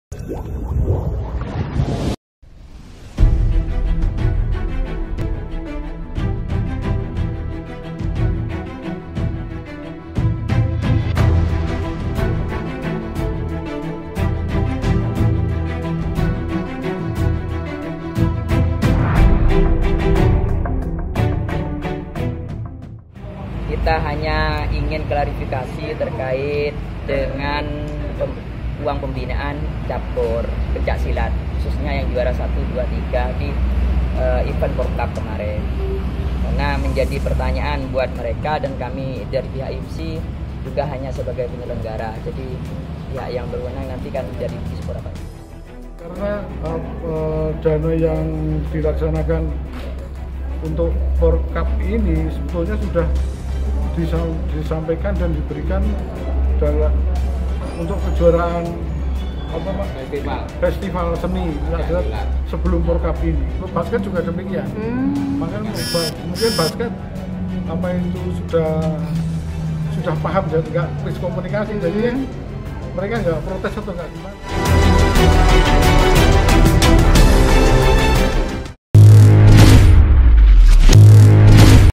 Tak Diberi Uang Pembinaan Pesilat Datangi Disporaparbud Kab. Probolinggo
Pesilat Kabupaten Probolinggo mendatangi kantor Dinas Pemuda Olahraga Pariwisata dan Kebudayaan Kabupaten Probolinggo, Kamis (22/11/2018). Mereka kecewa karena atlit dari IPSI saat Pekan Olahraga Kabupaten tidak mendapatkan uang pembinaan.